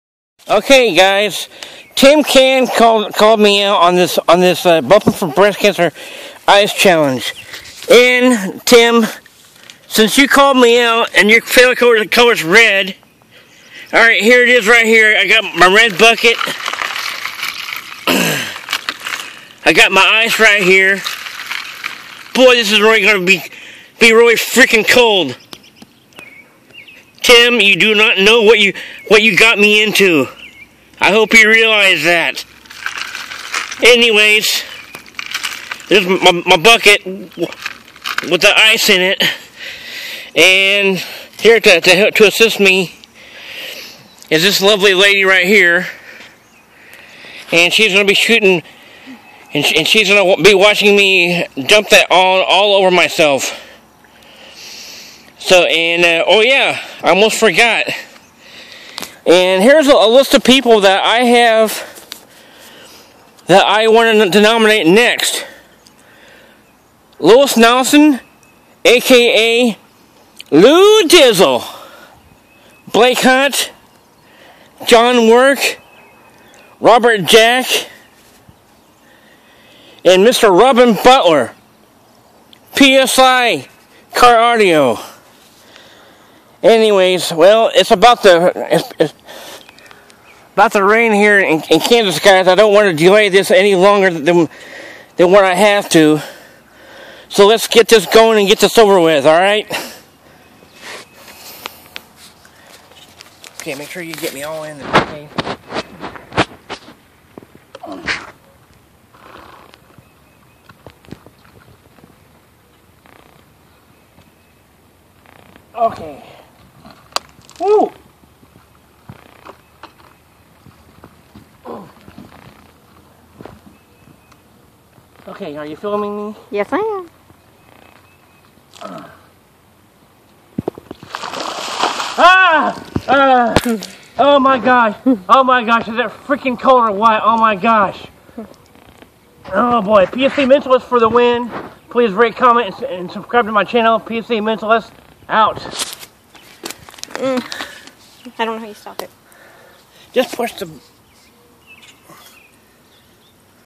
Bumpin 4 breast cancer ice challenge
If you guy wish not to accept you can always donate $100 to bumpin for breast cancer.